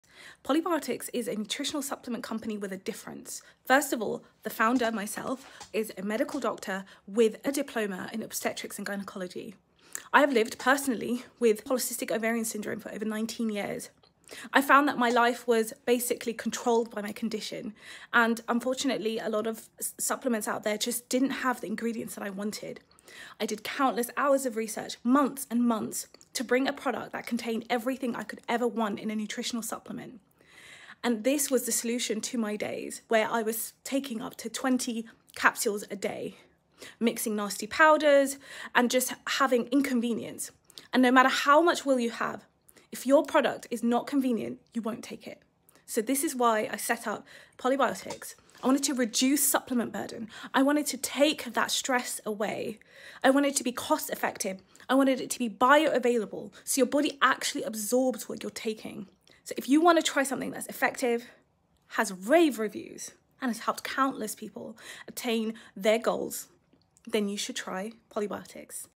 My PCOS Story: Why I Created Polybiotics | Dr Amina Hersi
As a medical doctor living with PCOS, I didn’t just want another supplement — I needed something that worked. I did the research and found inositol, a natural ingredient with real evidence behind it. But taking multiple supplements every day was exhausting, so I created a formula that simplified everything. Bioavailable, effective, and grounded in both science and lived experience.

This is more than a brand. It’s personal.

👩🏽‍⚕️ Founded by Dr Amina Hersi
💊 Science-led support for PCOS
🌿 Natural ingredients that actually work

📲 Follow @polybiotics for more PCOS content